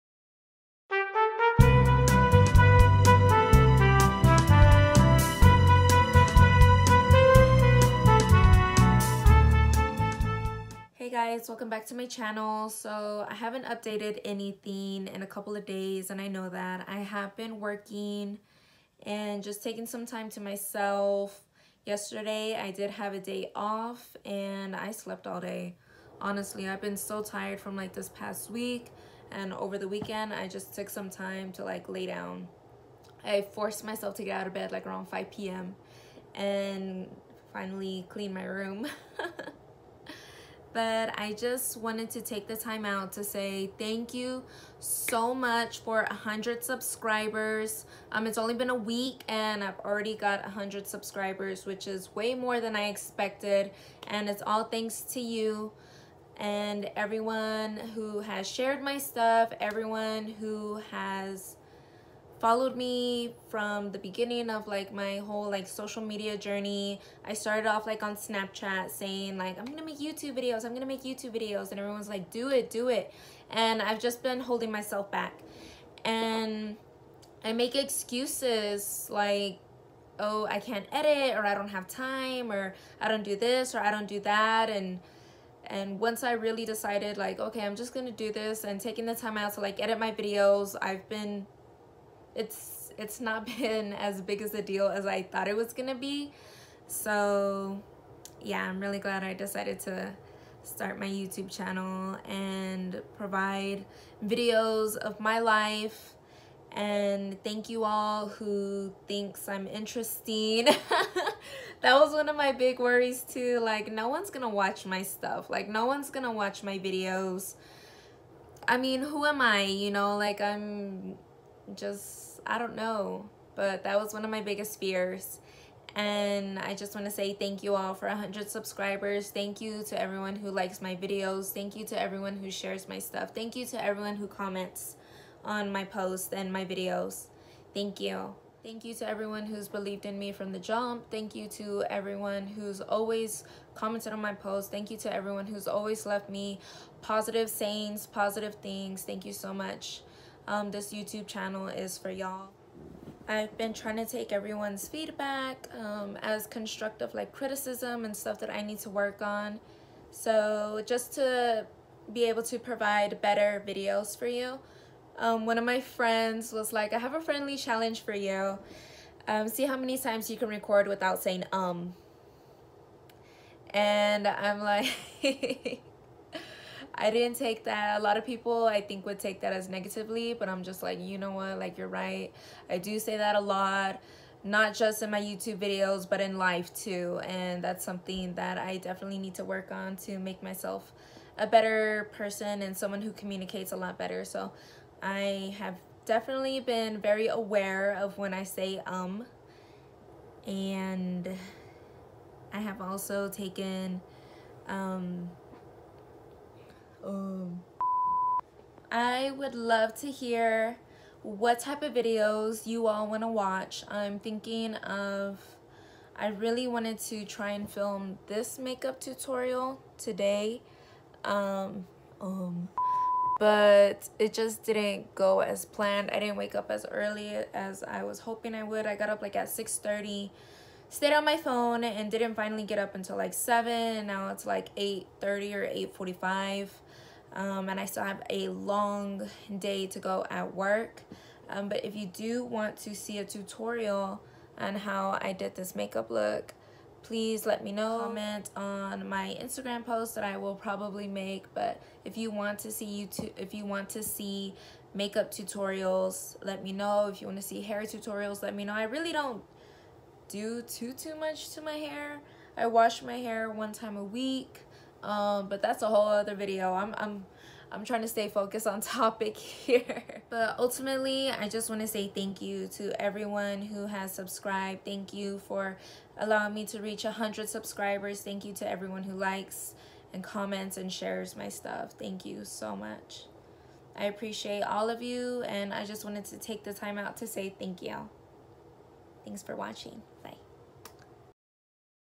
THANK YOU FOR 100 SUBS!
100 subscribers in a week is more than I ever expected! I just want to thank everyone for subbing, sharing, liking & commenting on my channel!